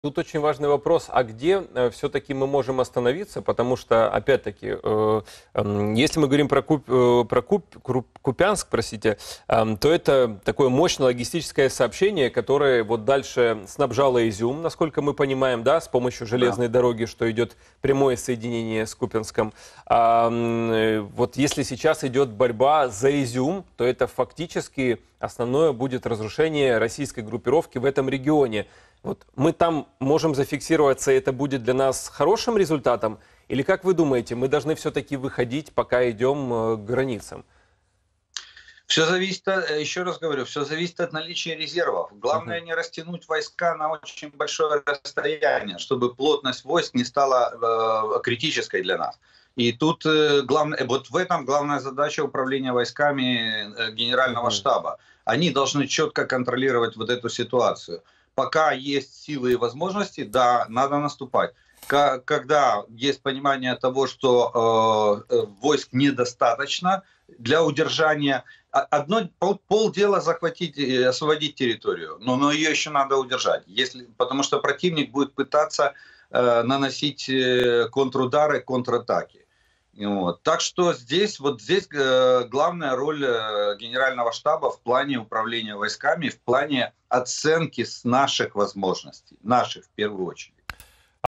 МЕДЛЕННОЕ УДУШЕНИЕ - тактика ВСУ на Херсонщине оккупантам не понравится
Интервью военным экспертом Олегом Ждановым записано на русском языке для телеканала FREEДОМ, который транслируется на русскоязычную аудиторию в военный период, чтобы донести правду о войне в Украине до зрителей из РФ.